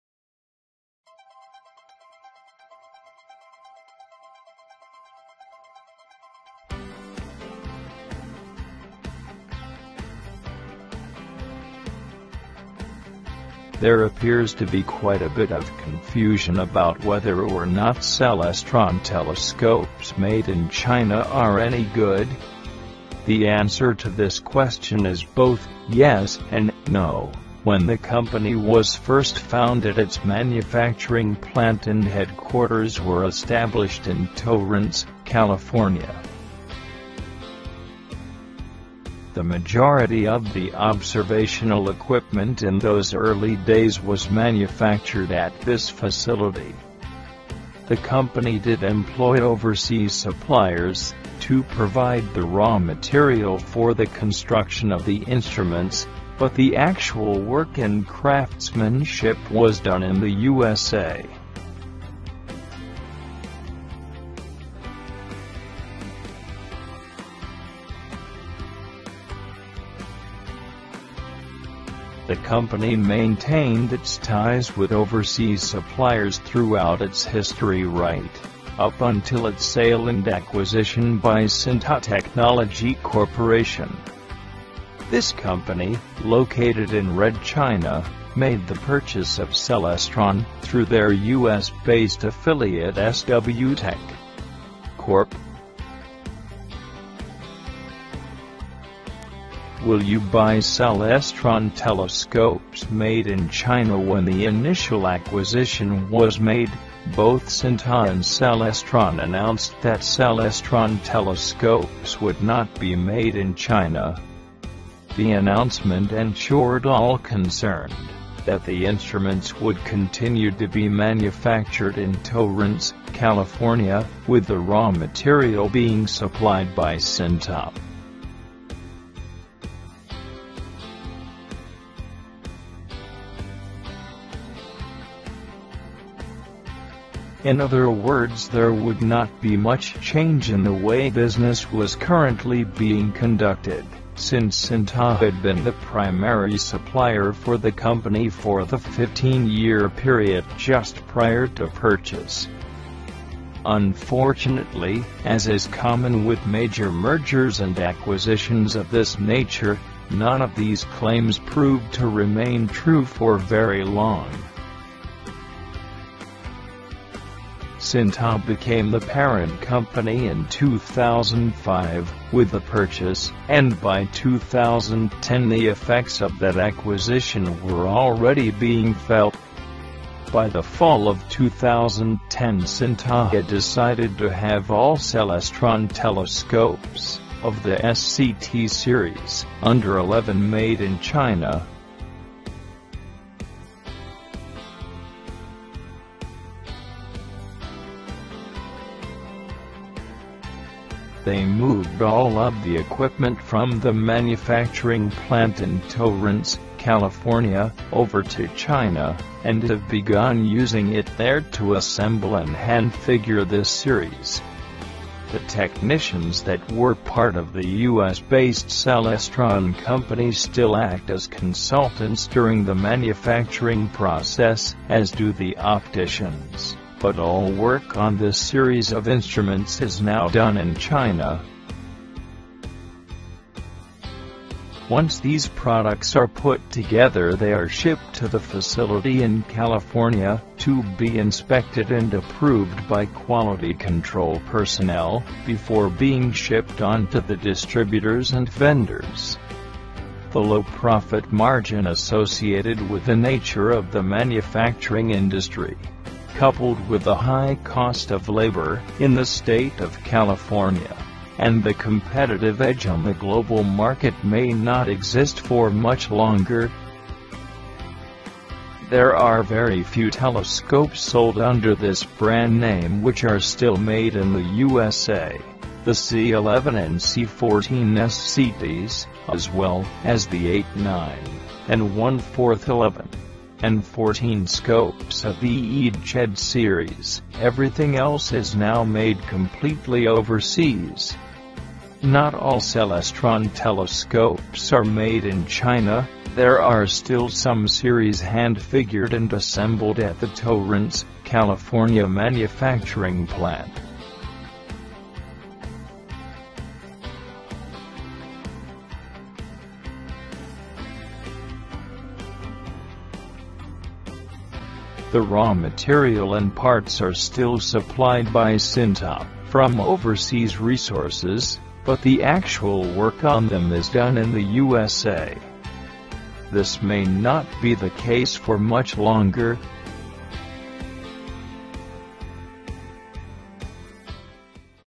Fine Review About Celestron Telescopes Made In China
There appears to be quite a bit of confusion about whether or not Celestron Telescopes Made In China are any good?  The answer to this question is both, 'yes' and 'no,' when the company was first founded its manufacturing plant and headquarters were established in Torrance, California.

The majority of the observational equipment in those early days was manufactured at this facility. The company did employ overseas suppliers to provide the raw material for the construction of the instruments but the actual work and craftsmanship was done in the USA.

The company maintained its ties with overseas suppliers throughout its history right up until its sale and acquisition by Synta Technology Corporation.  Corp.

Will You Buy Celestron Telescopes Made In China

When the initial acquisition was made, both Synta and Celestron announced that Celestron Telescopes would not be Made In China.  The announcement ensured all concerned that the instruments would continue to be manufactured in Torrance, California with the raw material being supplied by Synta.